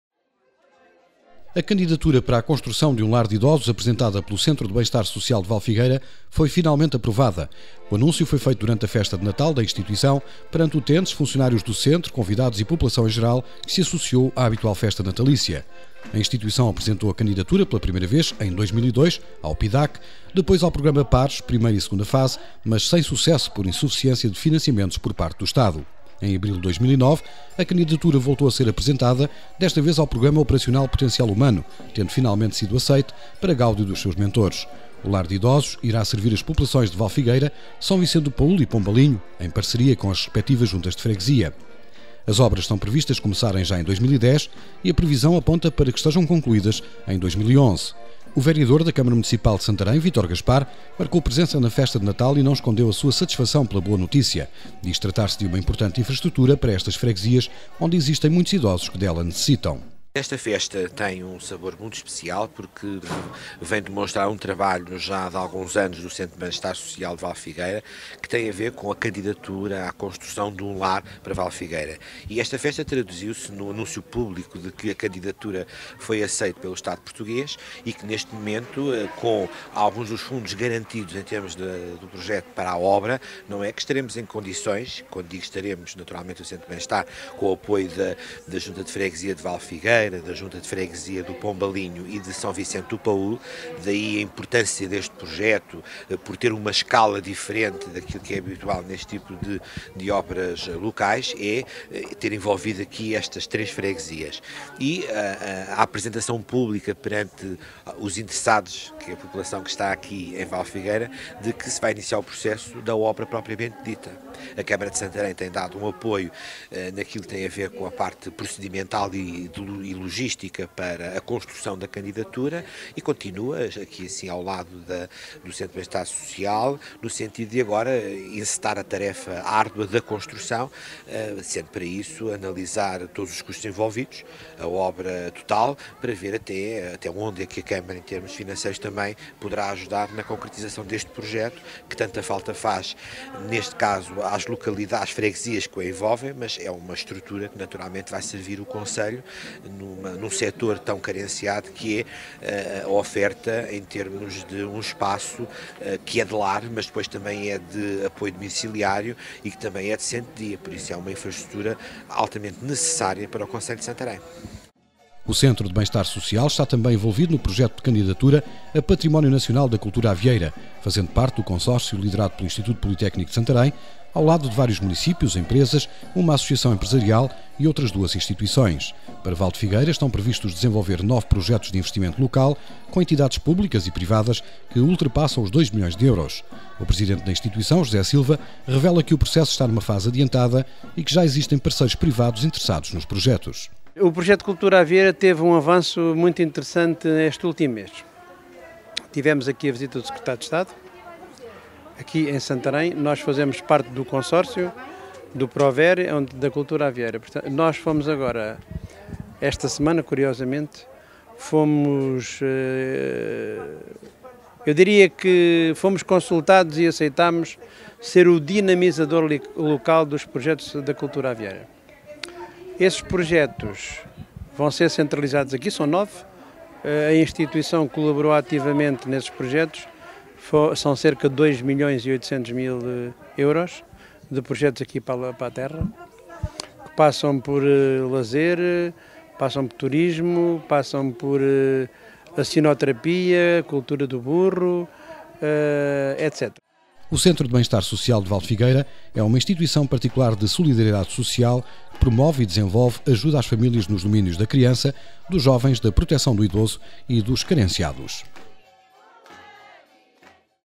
Aprovada candidatura para lar de idosos em Vale de Figueira
Equipamento  servirá Vale de Figueiro, S.Vicente do Paul e Pombalinho